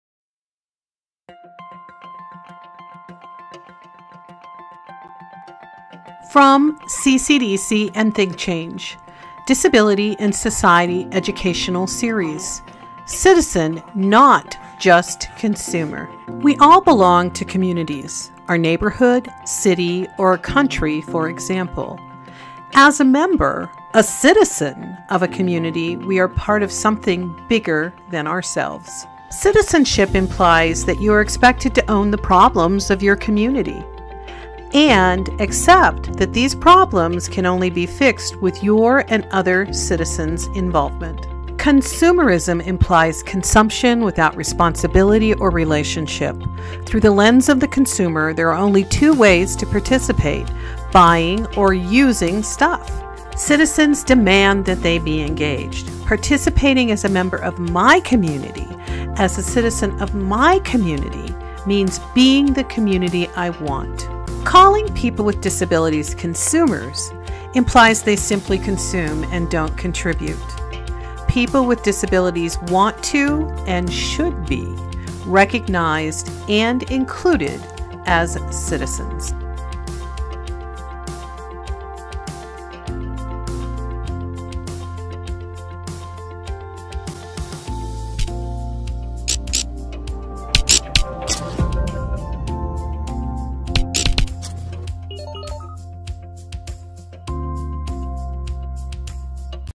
CCDC and Think+Change -Citizens Not Consumers
Citizens, not Consumers: (English Caption Available / Subtítulos en Español Disponibles)

We all belong to communities and as a member – a citizen -- of a community, we are part of something bigger than ourselves. Consumerism implies consumption without responsibility or relationship. People with disabilities want to and should be recognized and included as citizens.